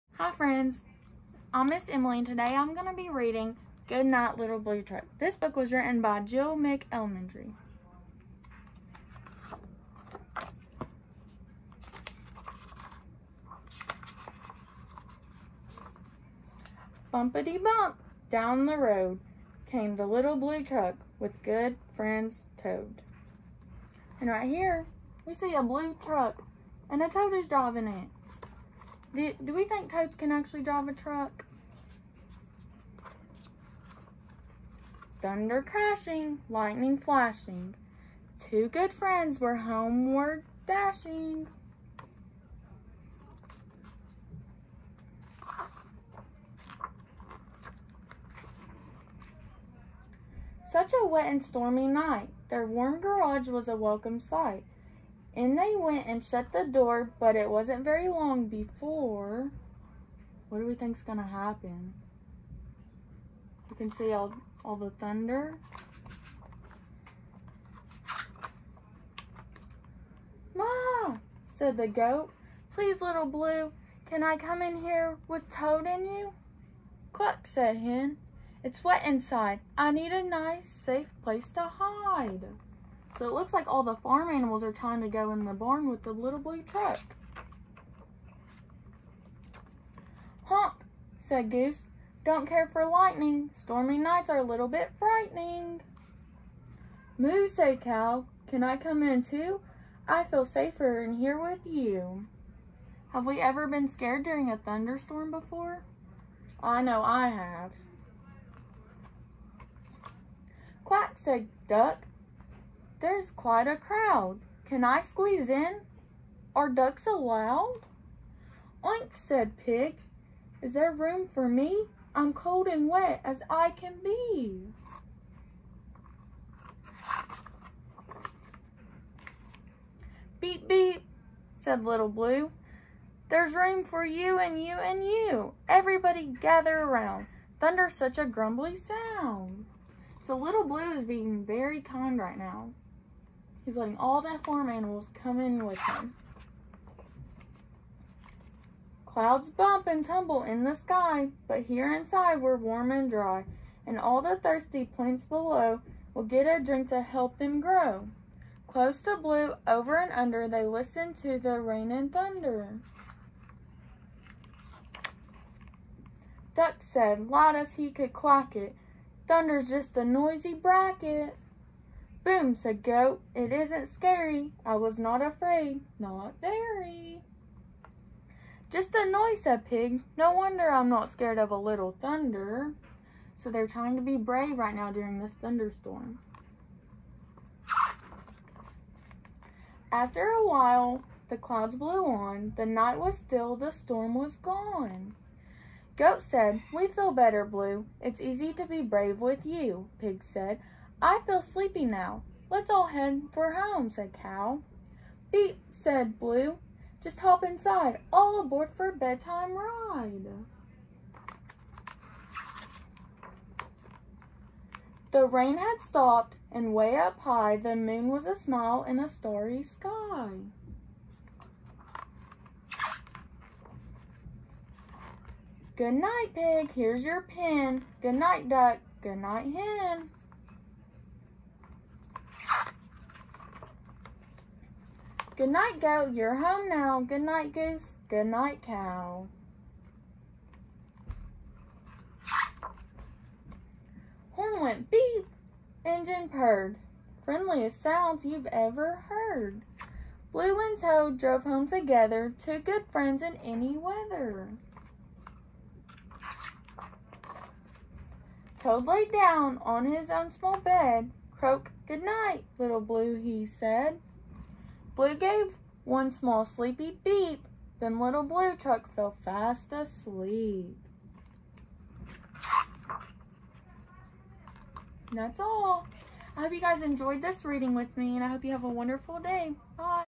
Good Night, Little Blue Truck Read Along
Join Ms. Emily as she reads Good Night, Little Blue Truck by Alice Schertle.